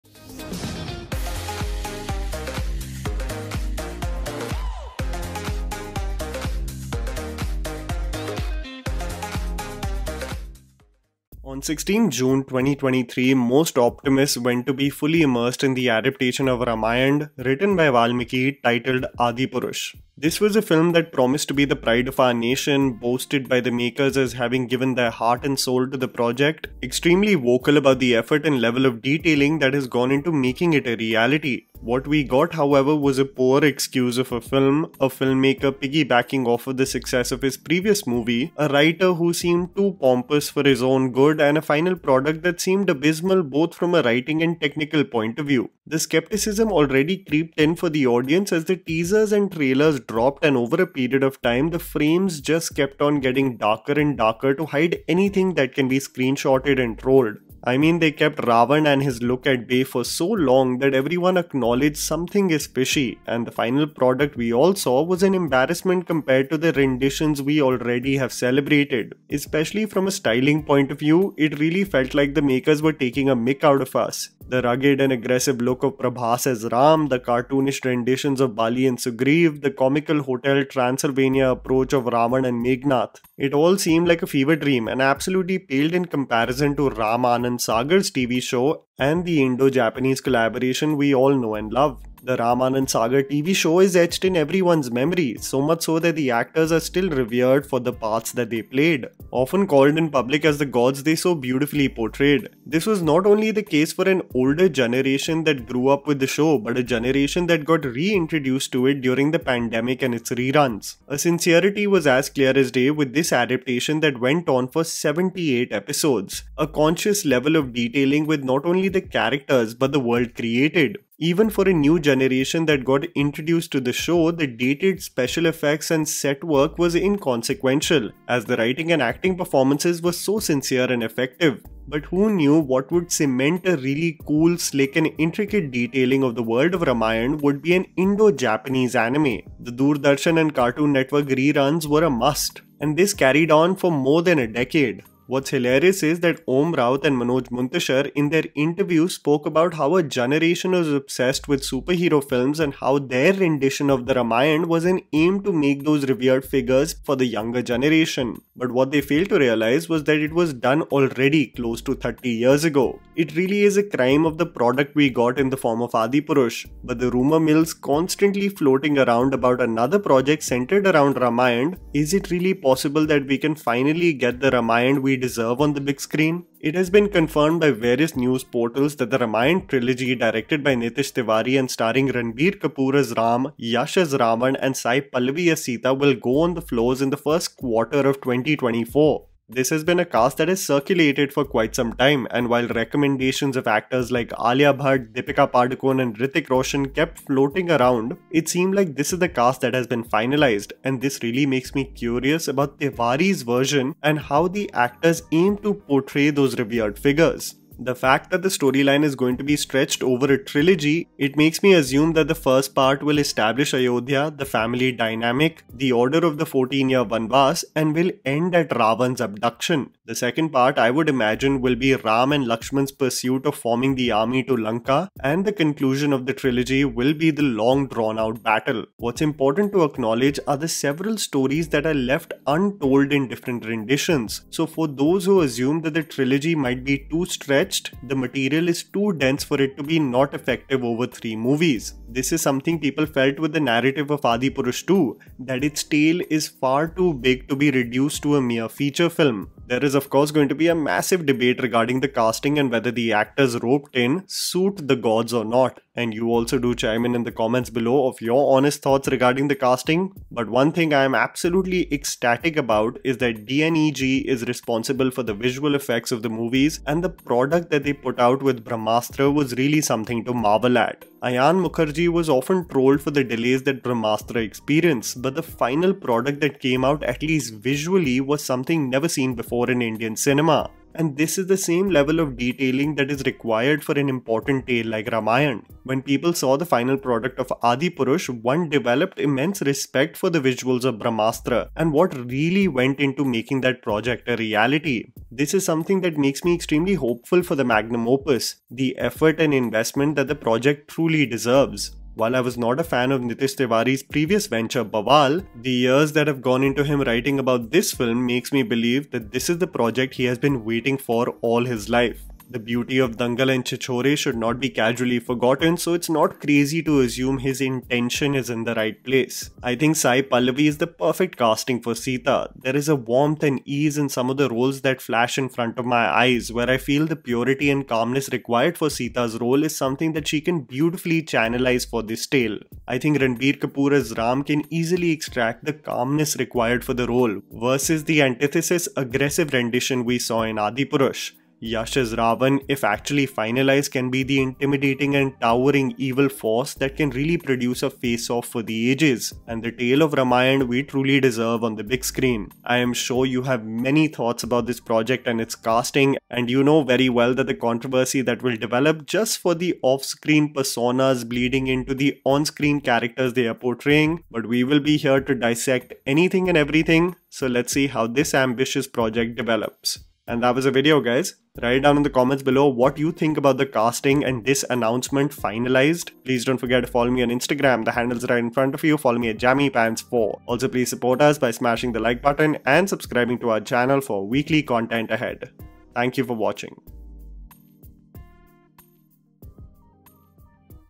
😱 Adipurush Will Be Forgotten | Ramayana Is Happening! | Ranbir Kapoor | Sai Pallavi | Yash
With ramayan presumably being approved and the cast finalised, will this ranbirkapoor saipallavi and yash starrer be the Ramayan we truly deserve! Directed by Nitesh Tiwari, will this bollywood project finally give the deserved respect to the sacred text. I hope you like the Ramayan Cast Announcement Review | Ranbir Kapoor | Sai Pallavi | Yash